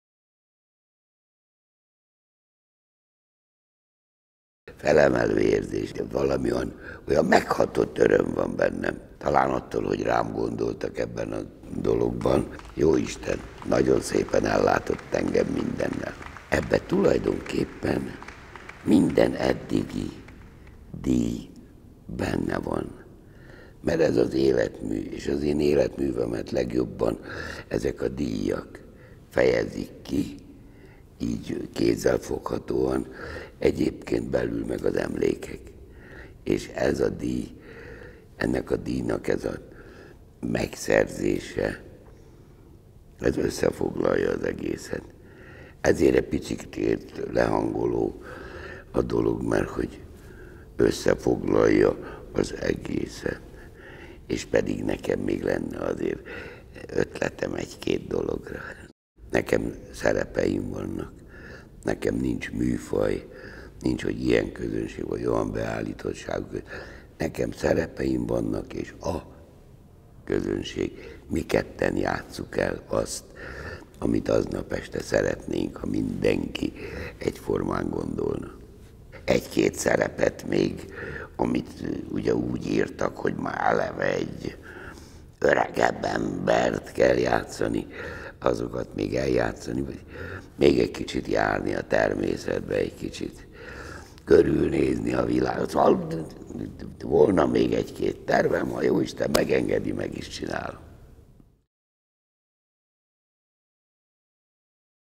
Bodrogi Gyula színművészé a Magyar Művészeti Akadémia Életműdíja 2019-ben
A Magyar Művészeti Akadémia Életműdíját 2019-ben Bodrogi Gyula színművész kapta példaértékű életművéért, kiemelkedő munkásságáért. A díjban a magyar szellemi élet azon jeles művésze részesíthető, akinek kiemelkedő munkássága, életműve példa a nemzet felemelkedéséért, megmaradásáért végzett odaadó, önzetlen és a személyes bátorságot sem nélkülöző munkálkodásra.